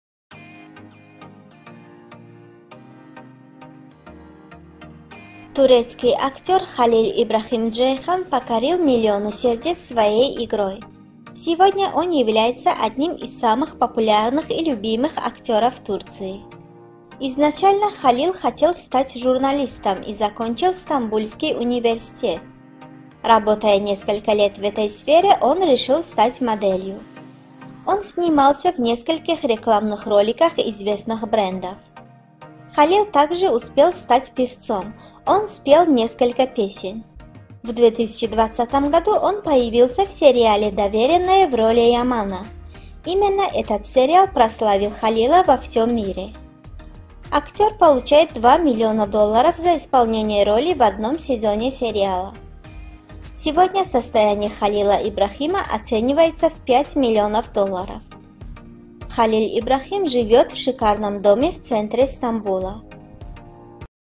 Как живет один из самых богатых актеров Турции Халиль Ибрахим Джейхан. Яман Доверенное Emanet
How one of the richest actors in Turkey Khalil Ibrahim Ceyhan lives. Yaman from the TV series Trusted

Turkish actor Khalil Ibrahim Ceyhan won millions of hearts with his performance. Today he is one of the most popular and beloved actors in Turkey. Initially, Khalil wanted to become a journalist and graduated from Istanbul University. After working for several years in this field, he decided to become a model. He starred in several commercials for famous brands. Khalil also managed to become a singer and sang several songs. In 2020, he appeared in the TV series Trusted as Yaman. It was this series that glorified Khalil all over the world. The actor receives $ 2 million for playing the role in one season of the series. Today, Khalil Ibrahim Ceyhan's fortune is estimated at $5 million. Khalil Ibrahim lives in a luxurious house in the center of Istanbul.

Cómo vive uno de los actores más ricos de Turquía, Khalil Ibrahim Ceyhan. Yaman de la serie de televisión Trusted

El actor turco Khalil Ibrahim Ceyhan ganó millones de corazones con su actuación. Hoy es uno de los actores más populares y queridos de Turquía. Inicialmente, Khalil quería convertirse en periodista y se graduó de la Universidad de Estambul. Después de trabajar durante varios años en este campo, decidió convertirse en modelo. Protagonizó varios comerciales para marcas famosas. Khalil también logró convertirse en cantante y cantó varias canciones. En el 2020, apareció en la serie de televisión Trusted as Yaman. Fue esta serie la que glorificó a Khalil en todo el mundo. El actor recibe 2 millones de dólares por interpretar el papel en una temporada de la serie. Hoy, la fortuna de Khalil Ibrahim Ceyhan se estima en $ 5 millones. Khalil Ibrahim vive en una lujosa casa en el centro de Estambul.

Türkiye'nin en zengin oyuncularından Halil İbrahim Ceyhan nasıl yaşıyor? Yaman Güven dizisinden

Türk oyuncu Halil İbrahim Ceyhan, performansıyla milyonlarca kalp kazandı. Bugün Türkiye'nin en sevilen ve sevilen oyuncularından biridir. Halil önce gazeteci olmak istedi ve İstanbul Üniversitesi'nden mezun oldu. Birkaç yıl bu alanda çalıştıktan sonra model olmaya karar verdi. Ünlü markaların çeşitli reklamlarında rol aldı. Halil ayrıca bir şarkıcı olmayı başardı ve birkaç şarkı söyledi. 2020 yılında Güvenilen dizide Yaman rolünde yer almıştır. Halil'i tüm dünyada yücelten bu diziydi. Oyuncu, dizinin bir sezonunda oynadığı rol için 2 milyon dolar alıyor. Halil İbrahim Ceyhan'ın servetinin bugün 5 milyon dolar olduğu tahmin ediliyor. Halil İbrahim, İstanbul'un merkezinde lüks bir evde yaşıyor.

Как живет один из самых богатых актеров Турции Халиль Ибрахим Джейхан. Яман из сериала Доверенное

Турецкий актёр Халиль Ибрахим Джейхан покорил миллионы сердец своей игрой. Сегодня он является одним из самых популярных и любимых актёров Турции. Изначально Халиль хотел стать журналистом и закончил Стамбулский Университет. Работая несколько лет в этой сфере, он решил стать моделью. Он снимался в нескольких рекламных роликах известных брендов. Халиль также успел стать певцом и спел несколько песен. В 2020 году он появился в сериале "Доверенное" в роли Ямана. Именно этот сериал прославил Халила во всём мире. Актёр получает 2 миллиона долларов за исполнение роли в одном сезоне сериала. Сегодня состояние Халиль Ибрахим Джейхана оценивается в 5 миллионов долларов. Халиль Ибрахим живёт в шикарном доме в центре Стамбула.